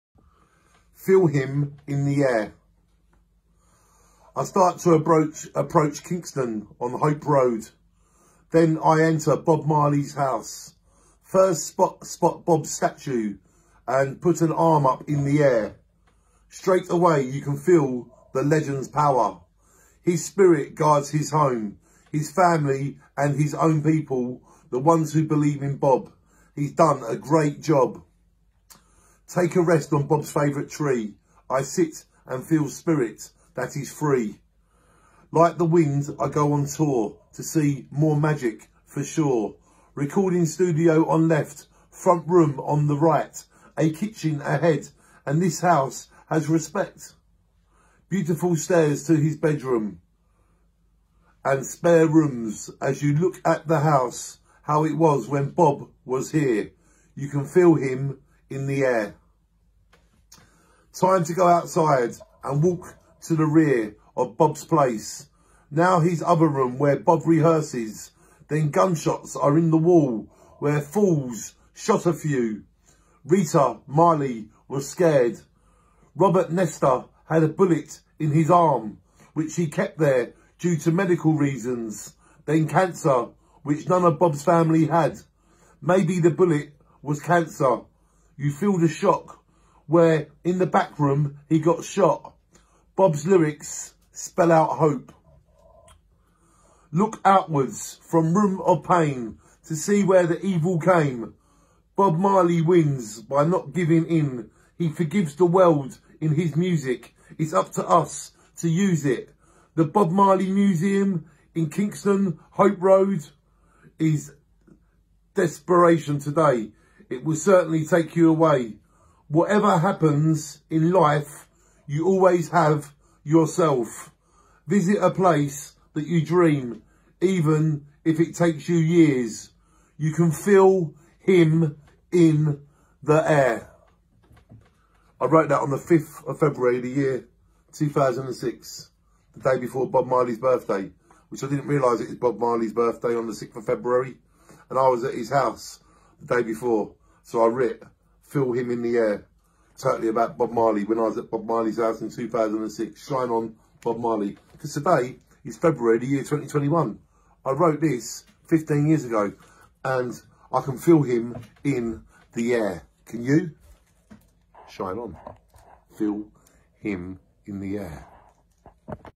Feel him in the air Bob Marley I’m in Jamacia on Bobs 60th Birthday so I wrote this for Bob 06 02 20
Feel him in the air (Bob Marley I’m in Jamacia on Bobs 60th Birthday so I wrote this for Bob)     06/02/2006.      Poem I wrote for Reallifedreamsmybook